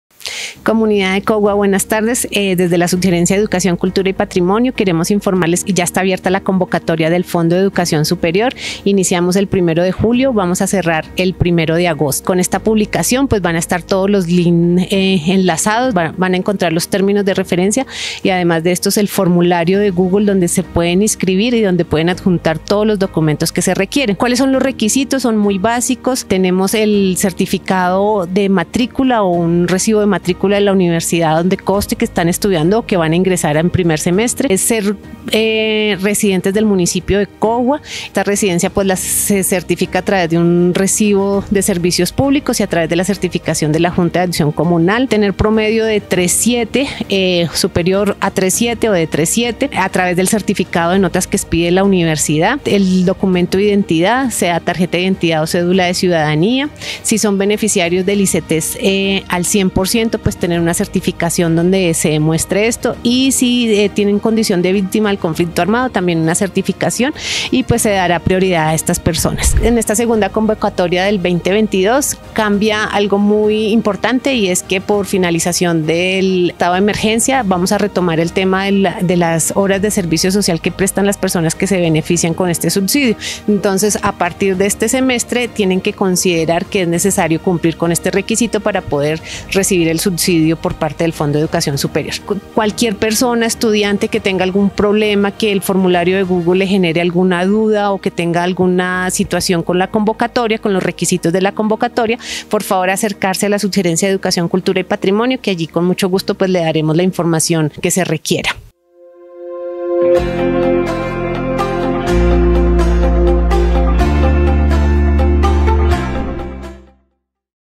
Fondo de Educación Superior
Educación || La Administración Municipal CoguaEnBuenasManos y la Subgerencia de Educación Cultura y Patrimonio Informan a la comunidad, que se da inicio a la convocatoria para acceder al Subsidio del Fondo de Educación Superior FES .